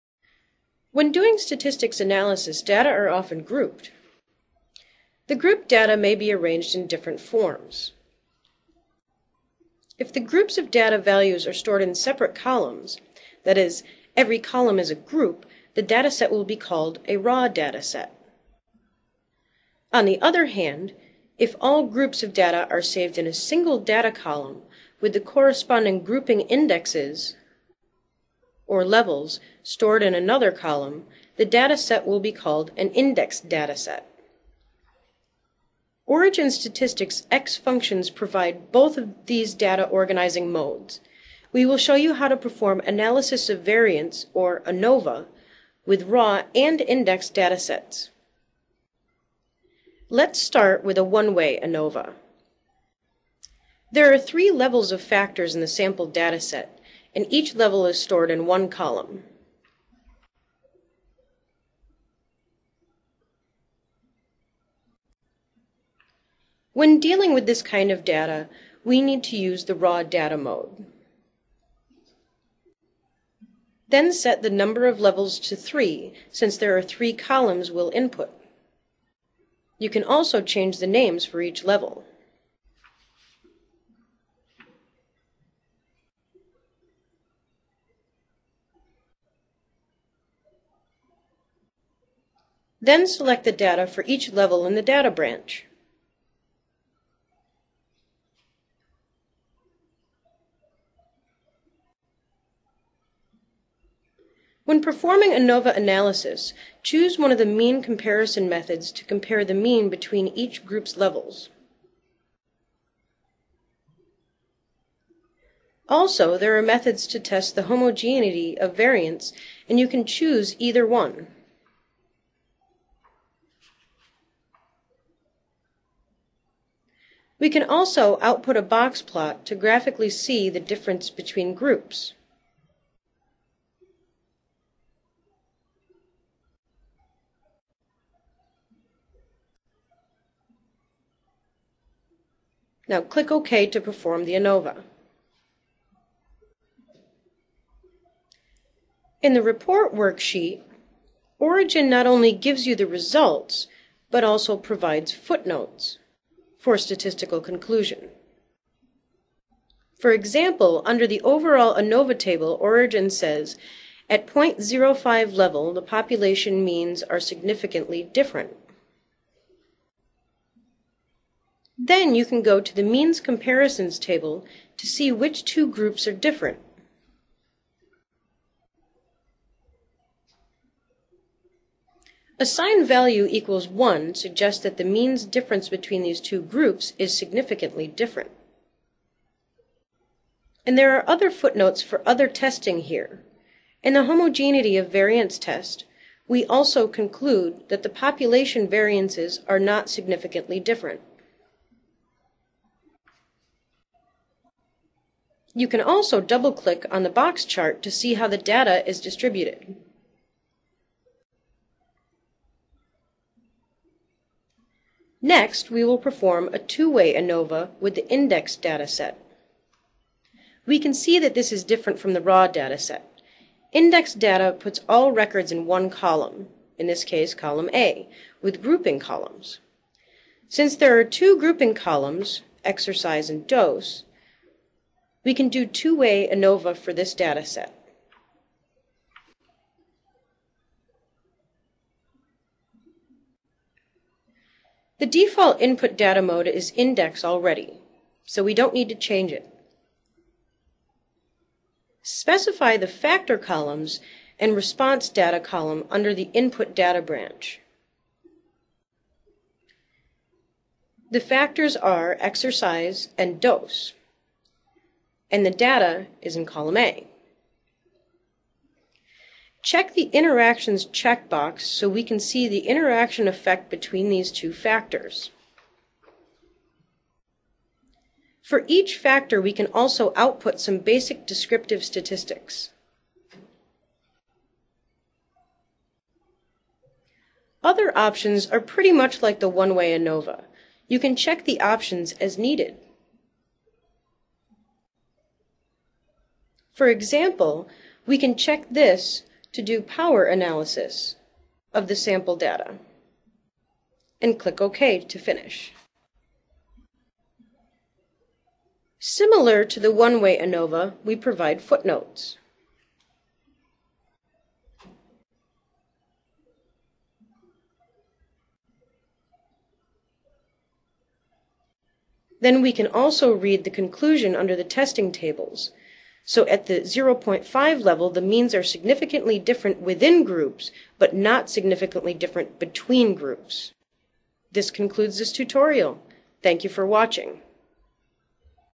Statistics: Origin 8.1: Analysis of Variance (ANOVA)
Learn how to perform One-Way and Two-Way Analysis of Variance (ANOVA) in Origin when your data are organized in two different ways.